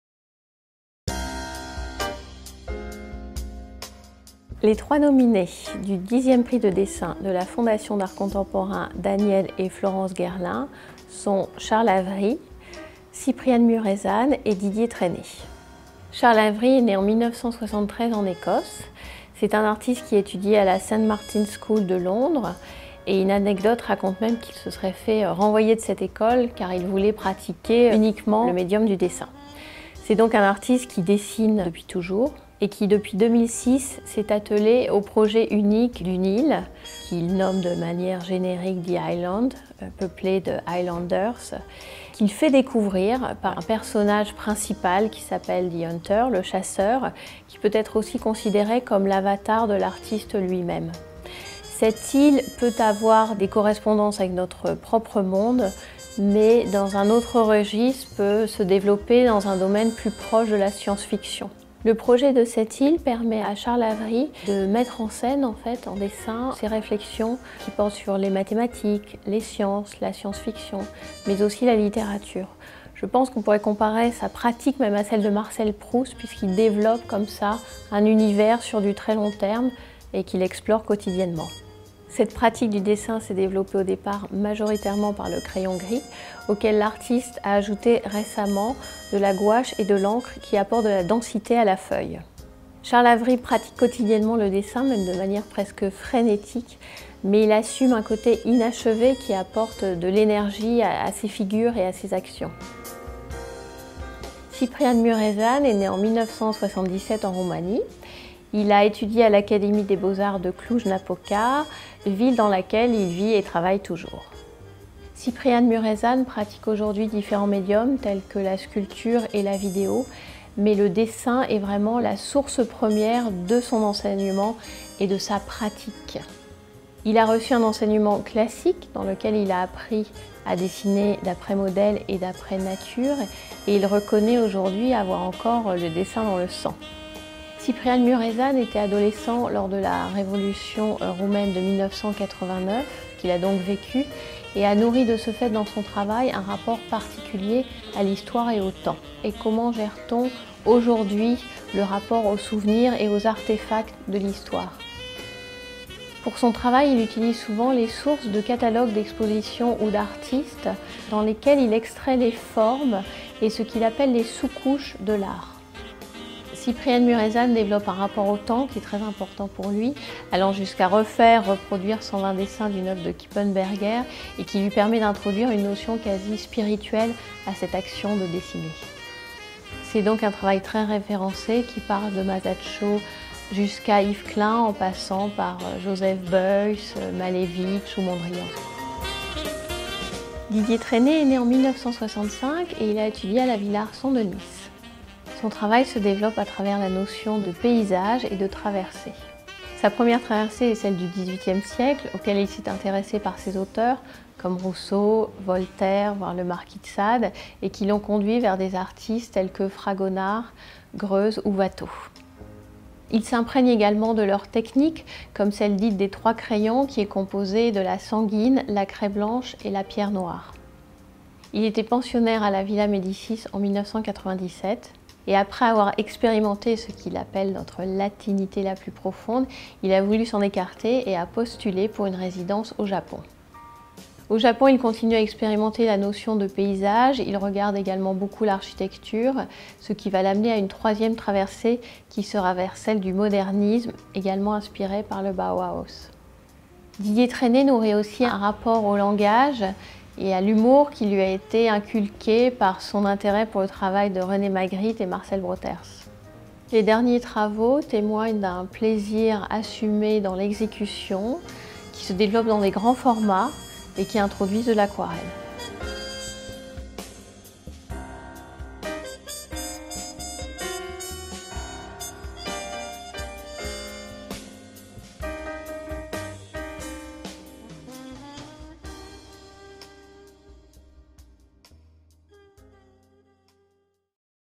Annonce des nominés du 10e Prix de dessin Guerlain
Marie Maertens journaliste et critique d'art, dévoile les trois artistes nominés du 10e Prix de dessin de la Fondation d'art contemporain Daniel et Florence Guerlain. Le lauréat du prix sera annoncé le 23 mars 2017 lors du Salon du dessin au palais Brongniart à Paris.

Réalisation : Maya Carillon, interview : Elodie de Dreux-Brézé